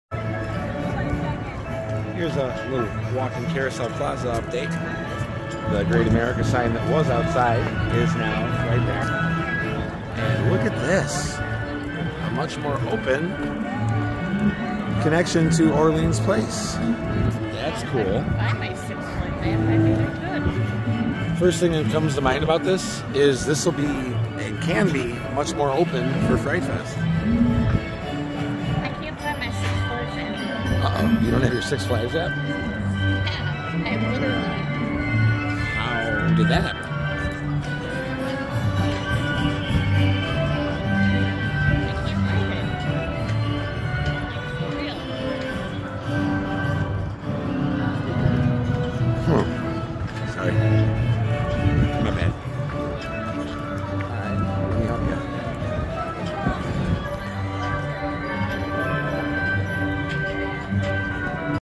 Carousel Plaza Updates Six Flags Great America Opening Day 4-20-24
Carousel Plaza has become much more open now that some trees have been cleared near the bathrooms, and a new path has to Orleans Place has opened up! Hopefully this eases congestion in this area. The sign from out front has also been moved in front of Columbia Carousel. The whole area has carousel music playing!